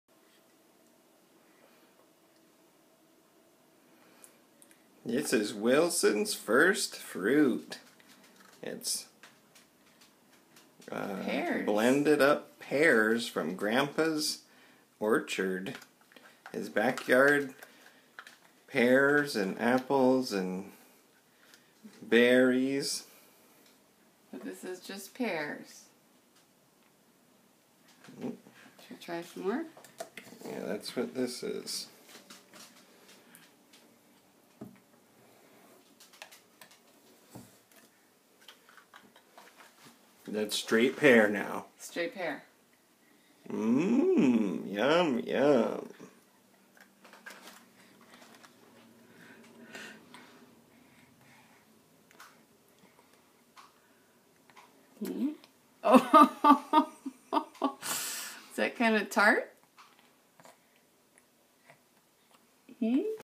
Wilson's first fruit
Pears from grandfather's backyard.  Almost 9 months